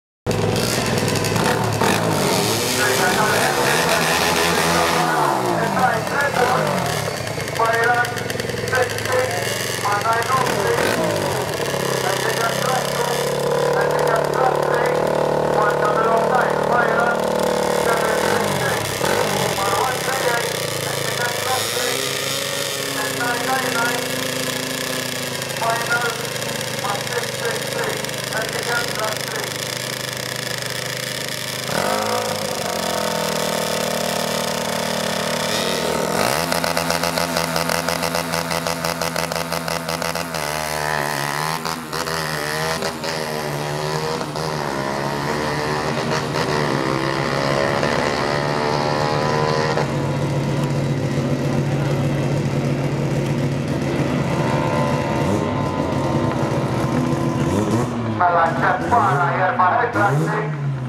November 2016 QTM Day 3 Run 130
27th November 2016 -  Day 3 "Eliminations and Finals" A great day of racing in this, the final event for 2016, which brought about many personal bests for many teams.

What a way to end the 2016 Season.

Time to take 3 months off before were back next Feb/March. See ya next year for the MDRA 2017 Drag Racing Series!